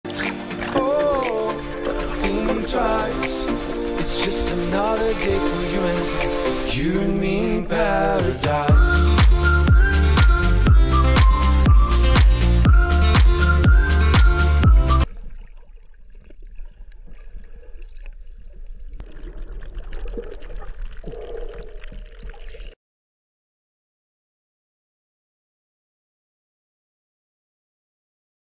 Swimming with our Green Sea Turtles 🏝️😍🩵
Swimming with our sea green turtles 😍

Journey with AboatTime is a Worthwhile Adventure - A Must!